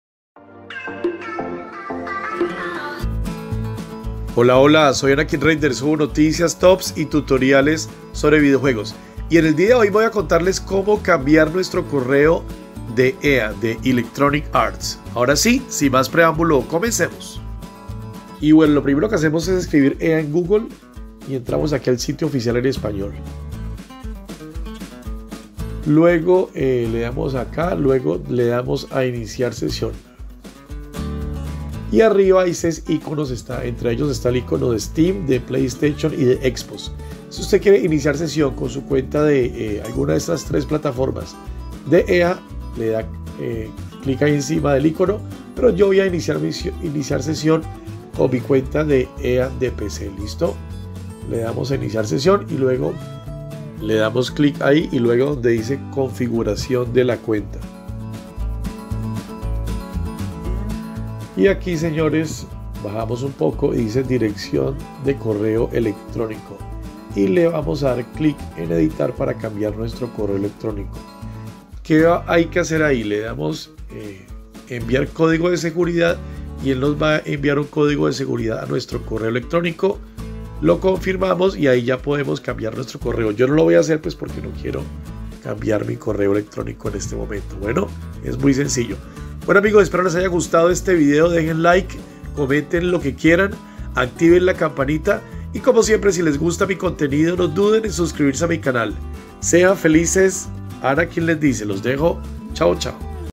Cómo Cambiar Correo Email de EA SPORTS
En este tutorial enseño como cambiar el email de ea sports